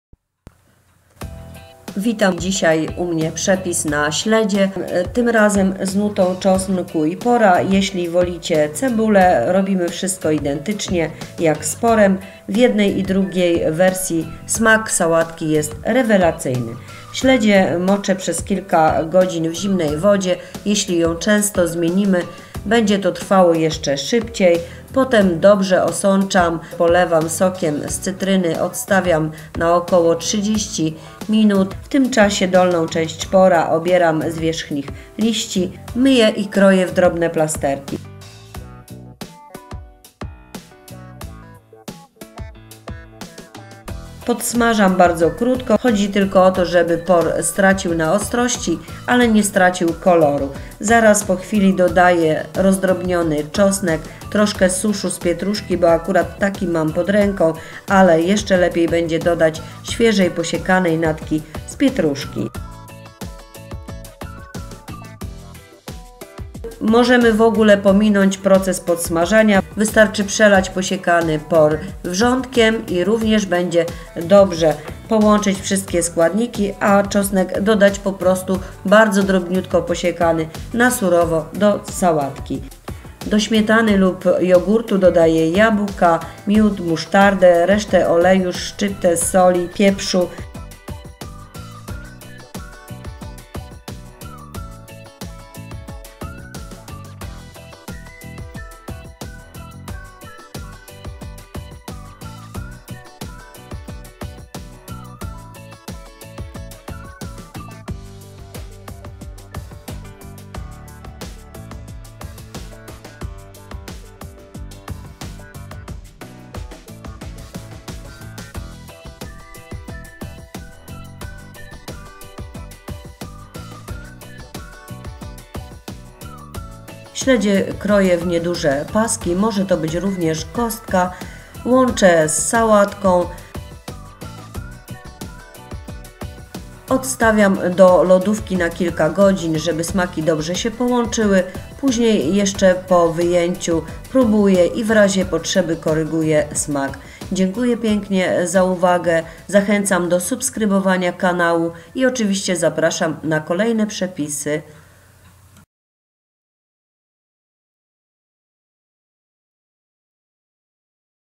Śledzie w sosie porowo-czosnkowym
Śledzie to jedno z najlepszych i najtańszych źródeł zdrowych tłuszczy na mądrość, odporność i humor. Polecam, jak zwykle prostą i bardzo pyszną przystawkę z ryby przynajmniej dwa-trzy razy tygodniu...